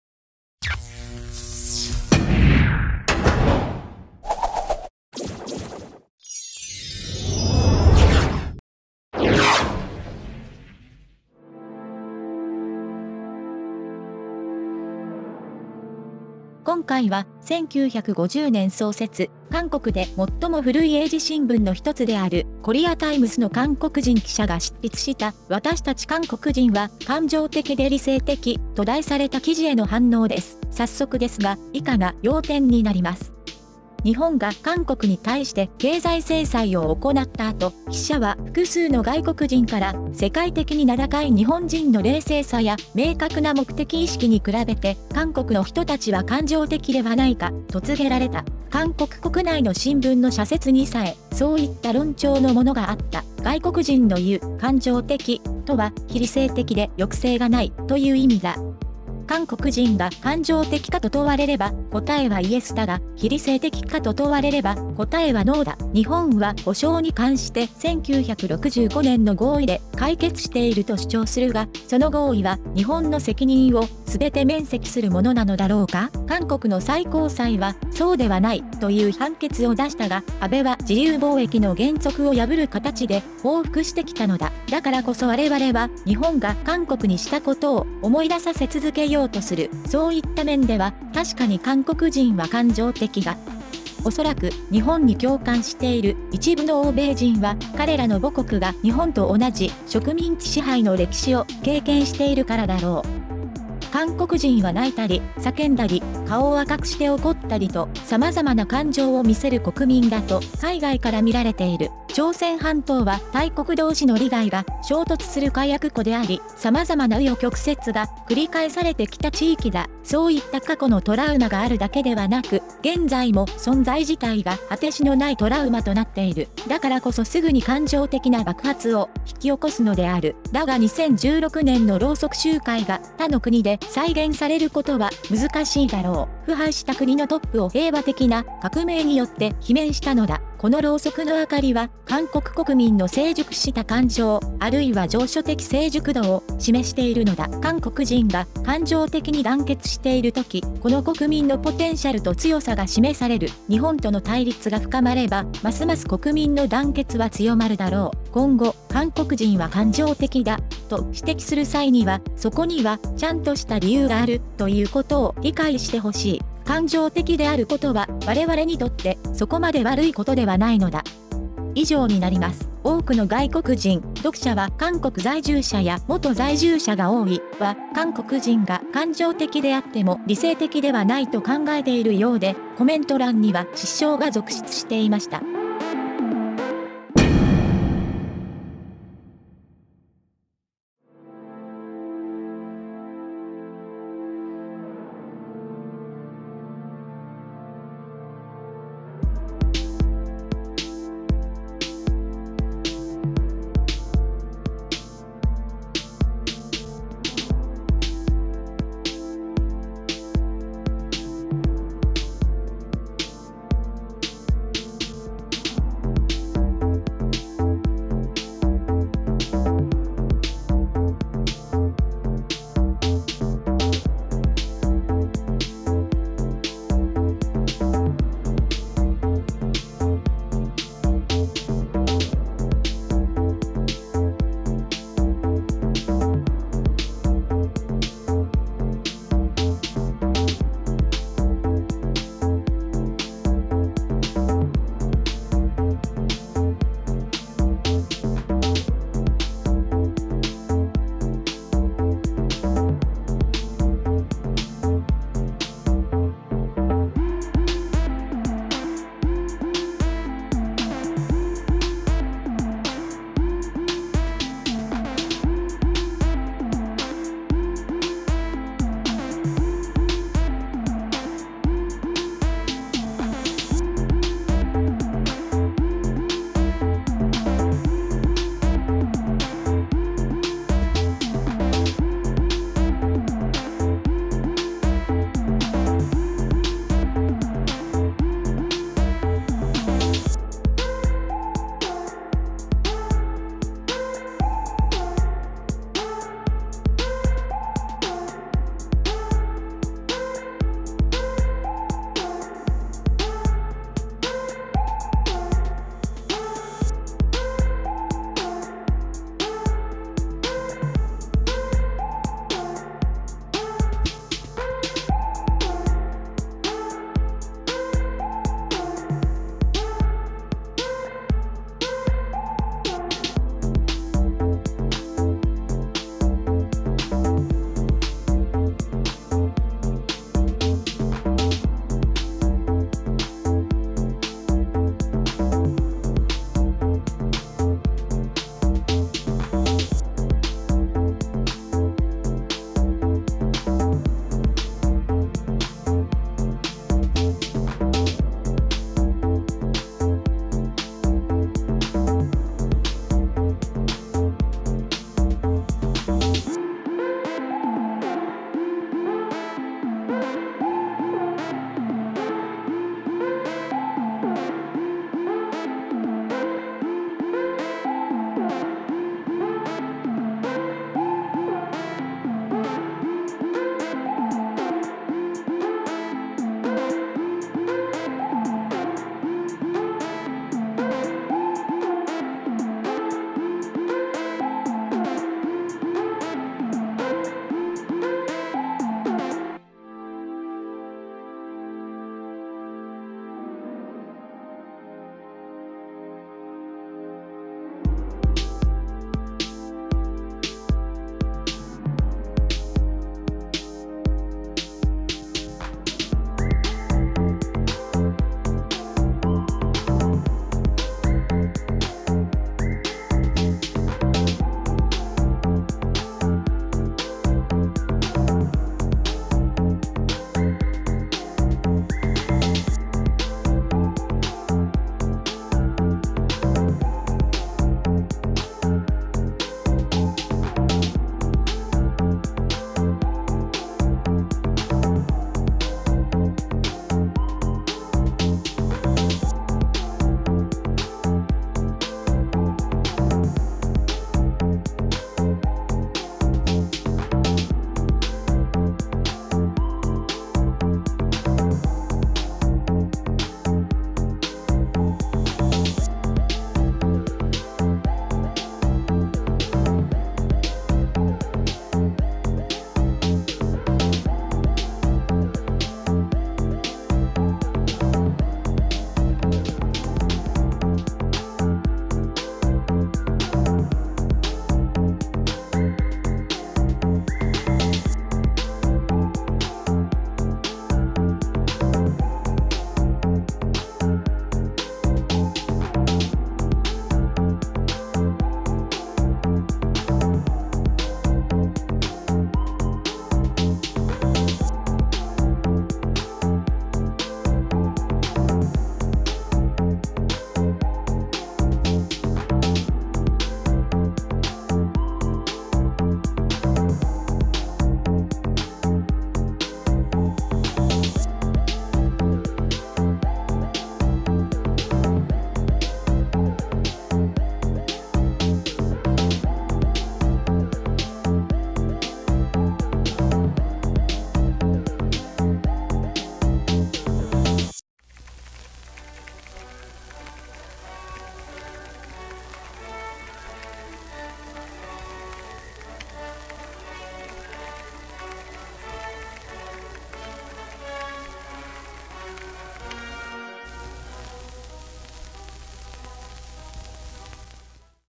韓国人が理性的だって？w日韓対立に関する韓国人記者の主張に知韓派も苦笑 海外の反応
韓国を感情的に全力擁護するタレントをデヴィ夫人が一撃でバッサリ！